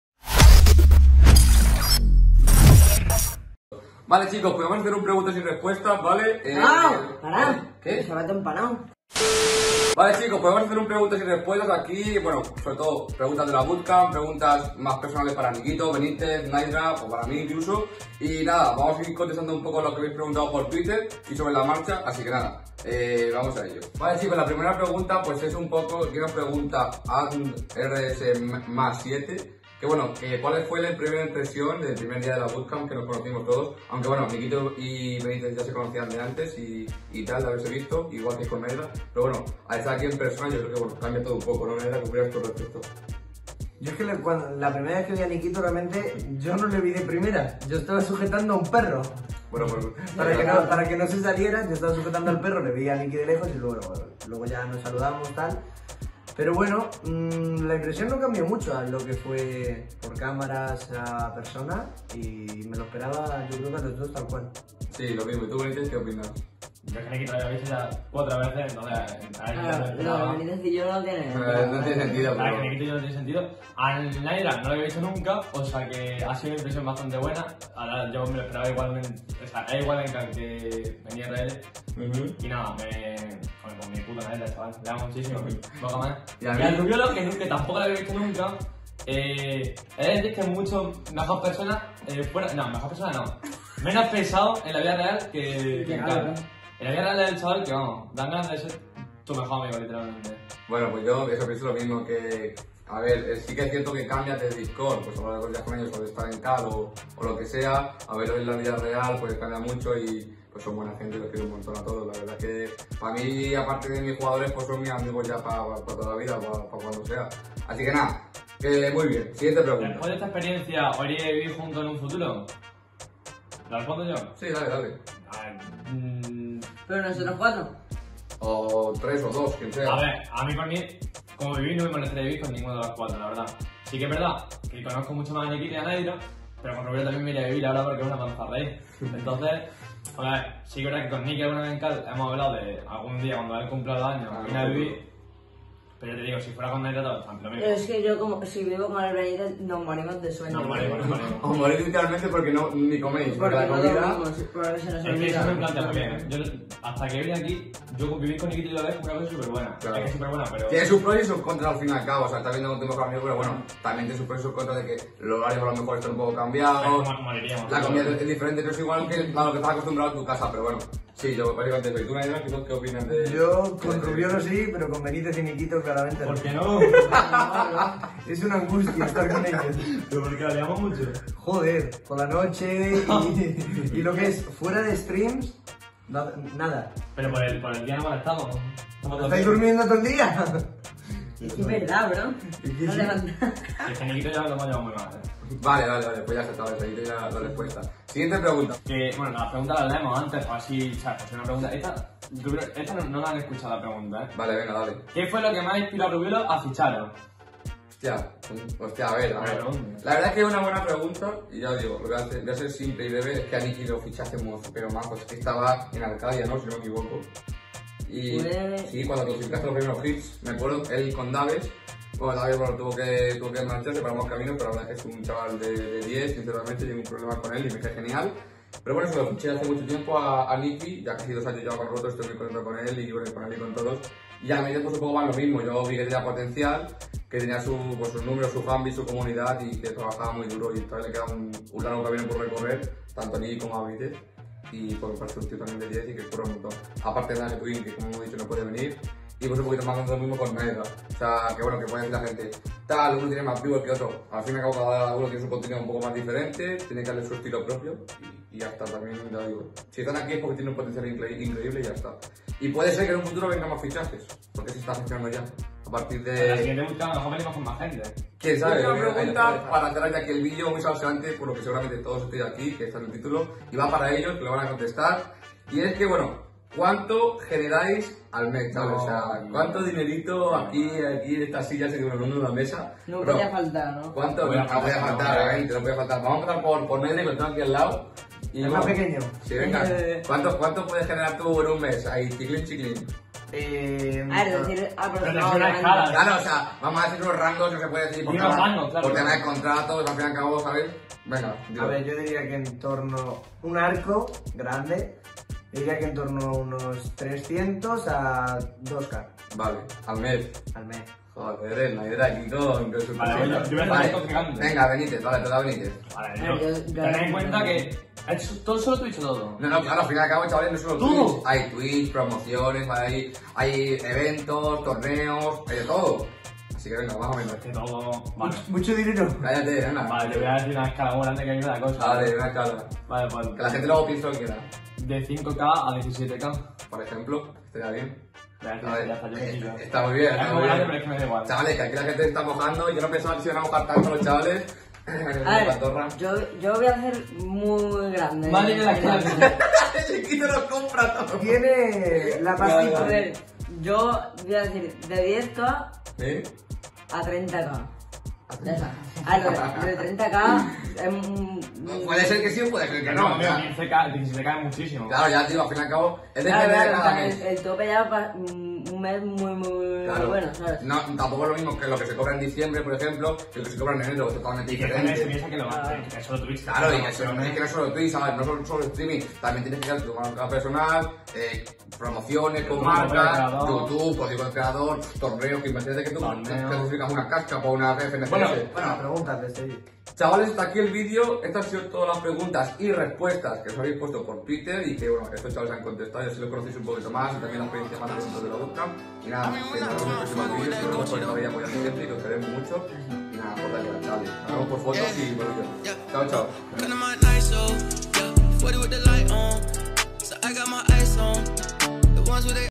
😱 ¿Cuánto DINERO ganáis en 1 mes? 😱 PyR - QLASH Fortnite | QLASH Spain.
- [PREGUNTAS Y RESPUESTAS QLASH FORTNITE] ¡Hoy os traemos un vídeo muy interesante de Preguntas y Respuestas con los chicos de FORTNITE, que realizamos en la QLASHBOOTCAMP!

- 🐉 QLASH Spain - eSports Club 🐉